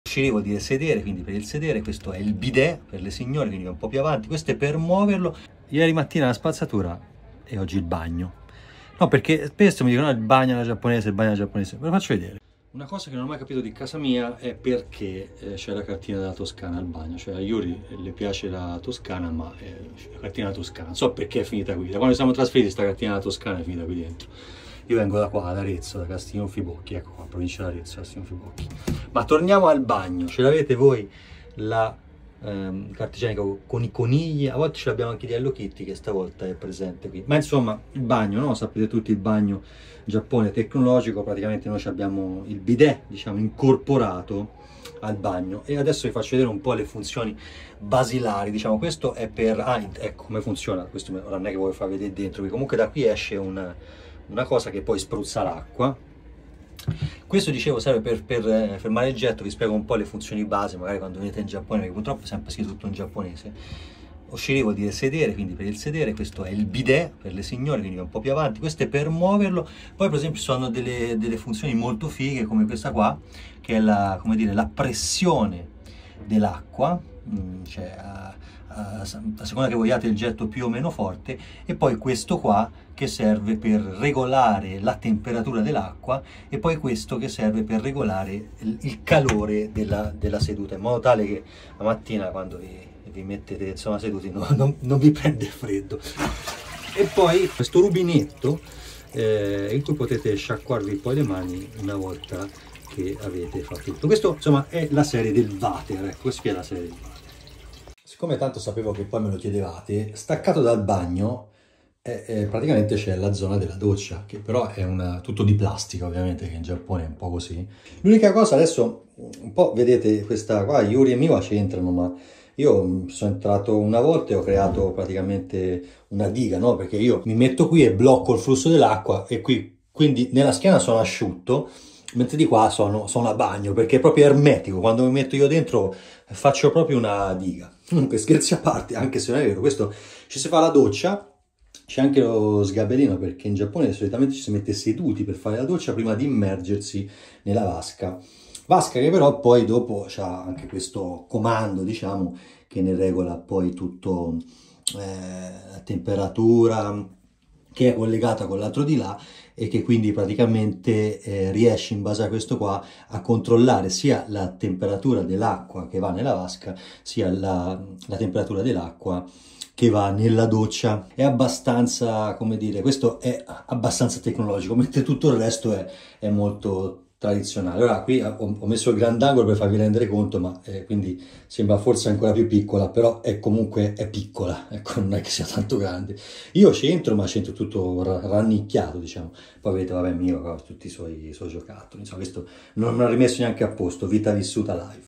IL BAGNO TECNOLOGICO IN GIAPPONE
Le funzioni del bidet unite alla tazza del bagno, ecco a voi il bagno tecnologico giapponese. Istruzioni per l'uso 🙂 P.s non ho mai capito perchè nel bagno di casa mia c'è la cartina della Toscana 🙂

TUTTE LE LIVE POTETE SEGUIRLE IN DIRETTA PARTECIPANDO IN CHAT SUL MIO CANALE TWITCH: